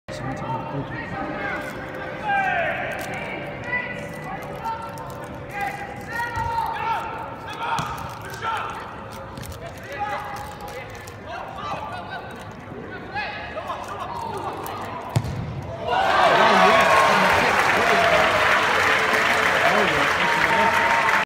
Noah Chesmain superb goal for Millwall vs Woodbridge Town FC
Noah Chesmain scored this superb goal for Millwall U18's vs Woodbridge Town in the FA Youth Cup which Millwall went on to win 9-1 on the night.